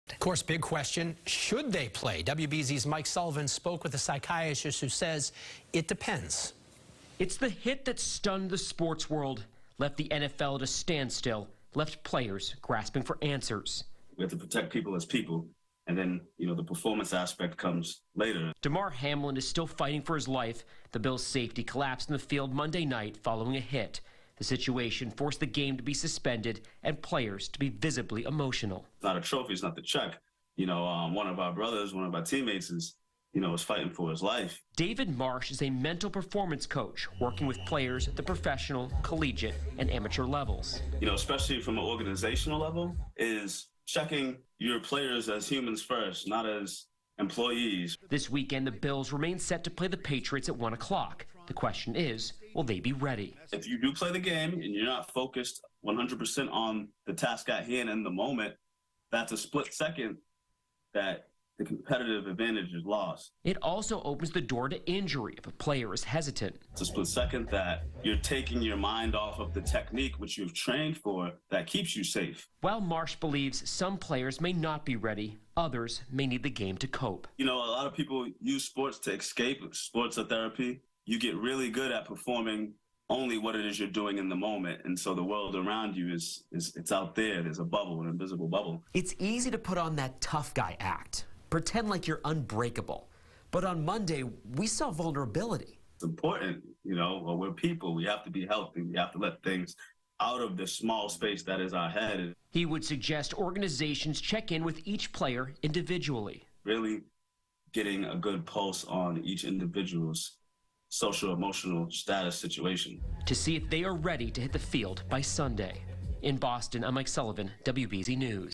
'We have to protect people as people.' Sports psychologist weighs in on whether Bills should play th
WBZ-TV's Mike Sullivan reports.